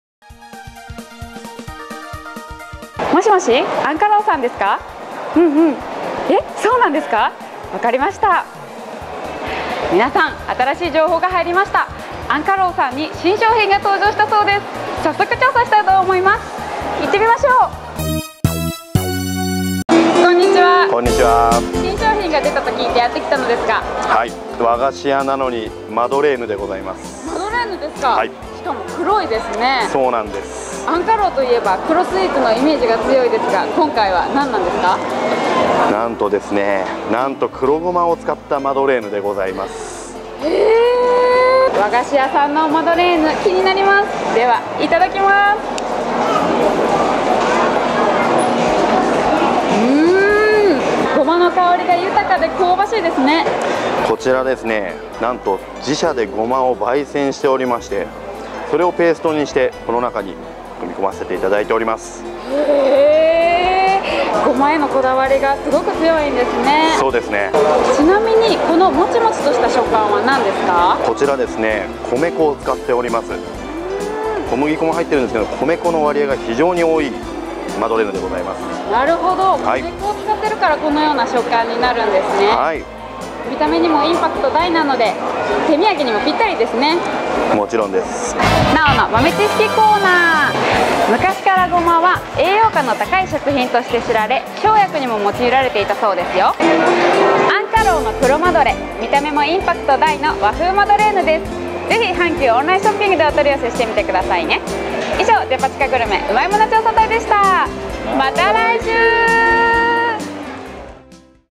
【阪急百貨店】うまいもの調査隊「餡菓桜」 黒まどれ
うまいものには理由がある。そんな逸品のこだわりをうまいもの調査隊がレポート。
デパ地下グルメを徹底調査！デパ地下グルメうまいもの調査隊。今回は、「餡菓桜」　黒まどれ

 阪急オンラインショッピングはこちら